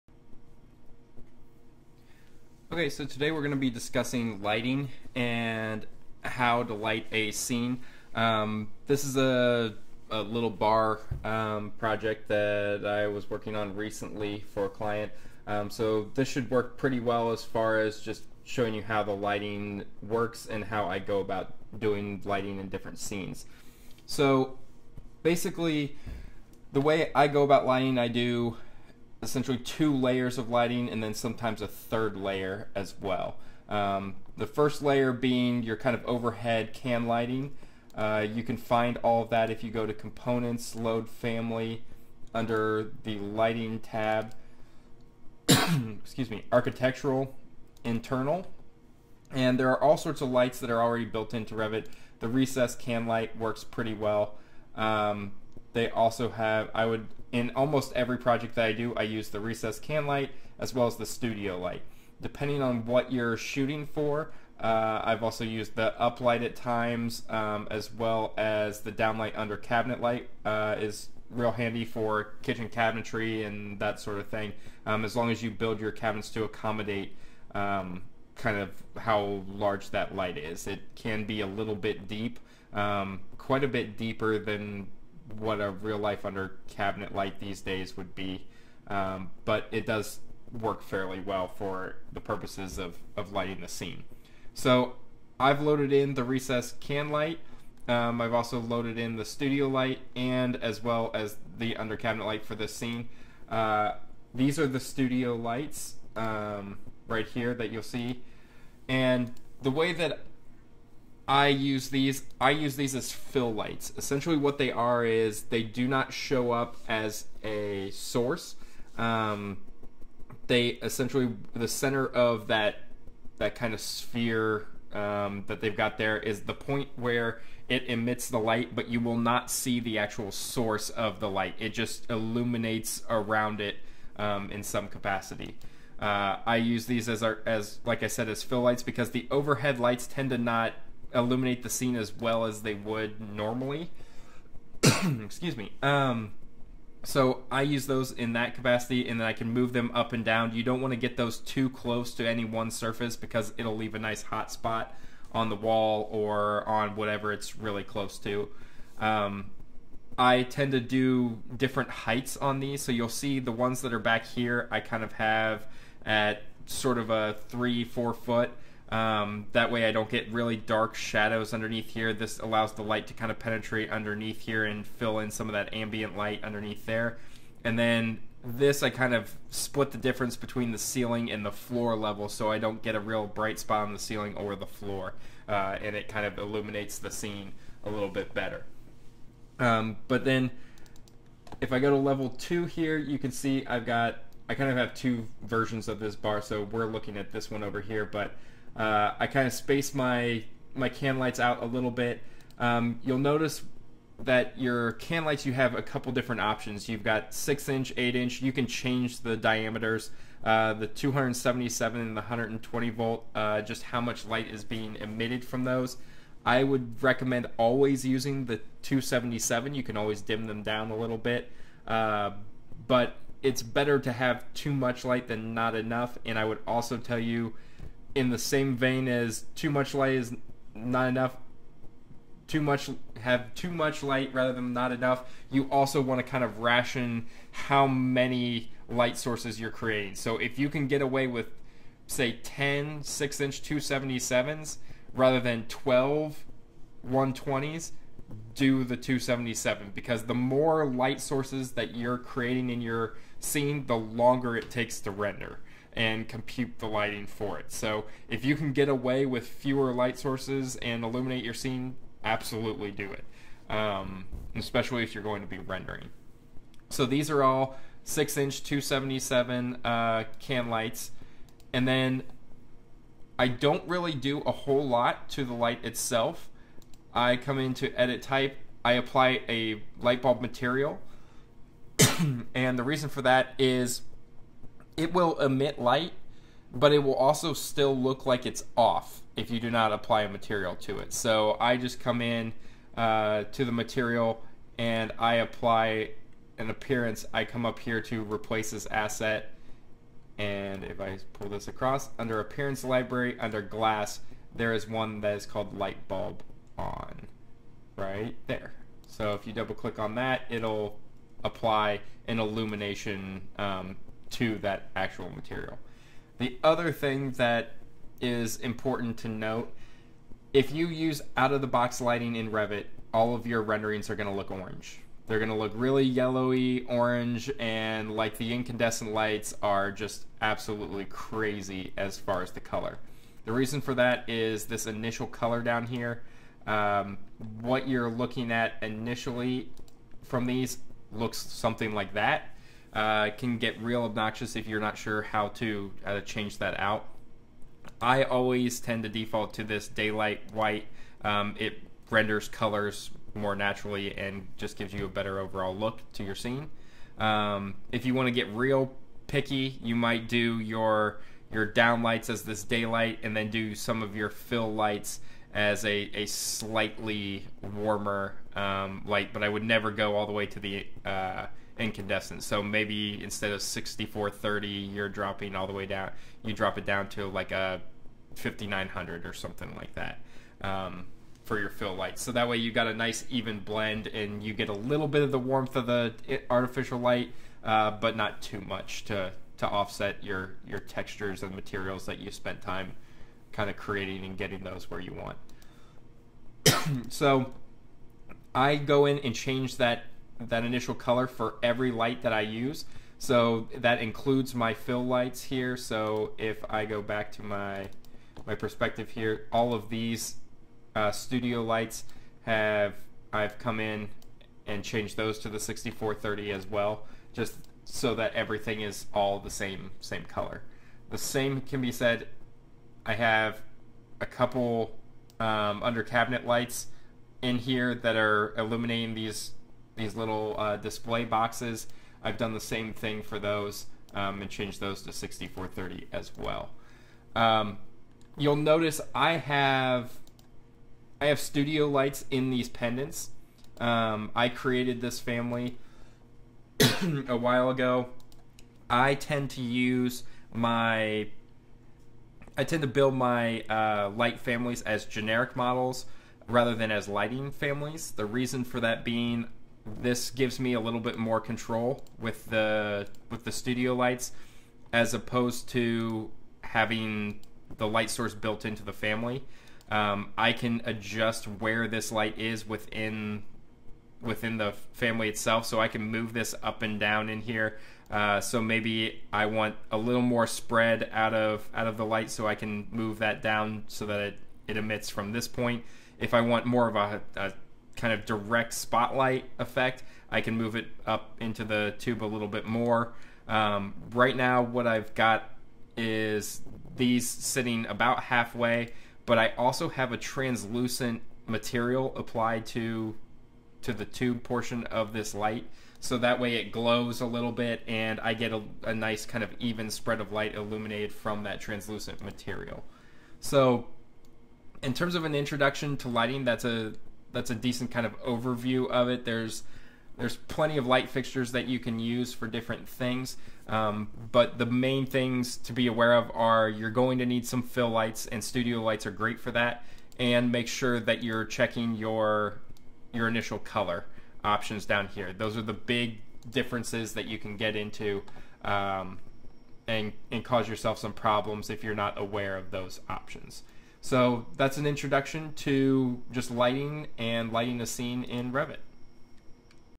Revit 2019 - Rendering - Episode 1 - Lighting
The basics of lighting a scene and the properties of lights in Revit that impact rendering.